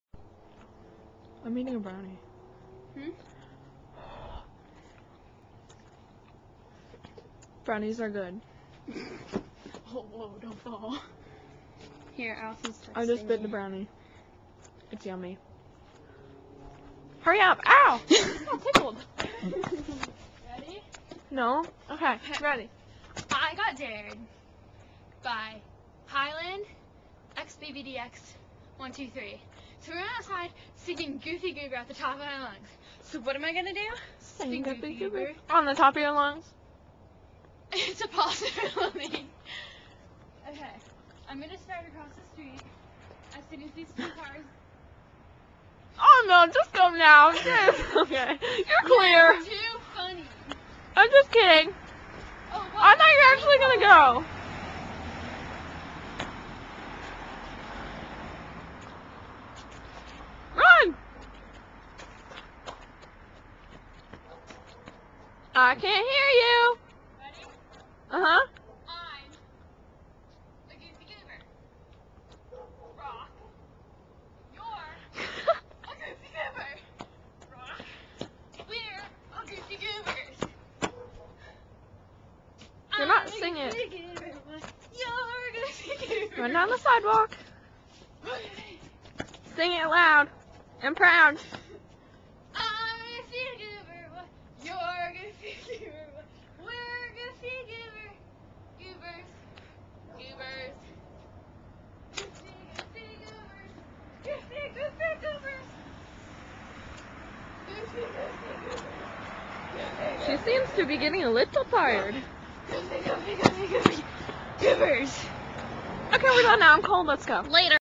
TRUTH OR DARE YOUTUBE STYLE!! :)
shpanks for the dare highlandxbbdx123!
CRAZYTUMBLE2 truth or dare ??? -evil eye-
if u want to join the game comment on this video or just message me or highlandxbbdx123 for directions on what to do :D